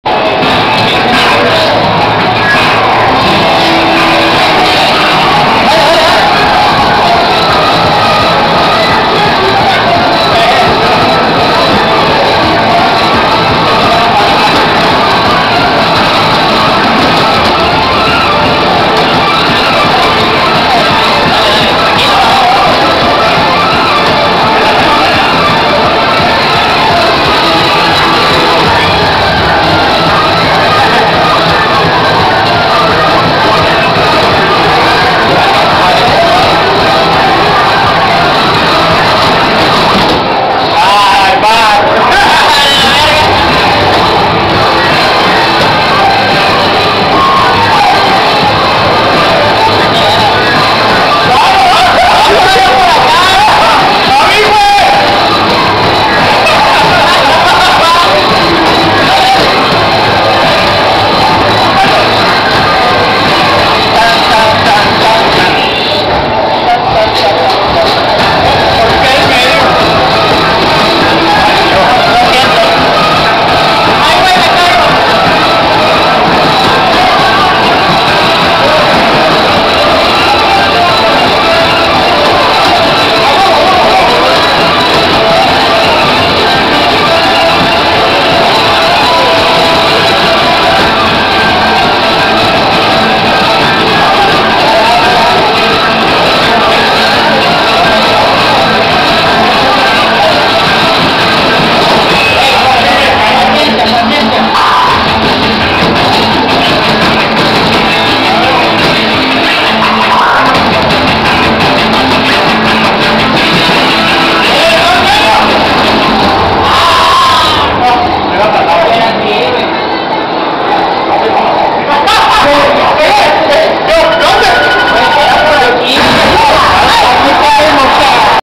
Take out Triple Performance
En este video jugando Take Out triple performace y el Mario se pasa de lanza le valio y se pasa por arriba cuando era por abajo que pedo con ese wey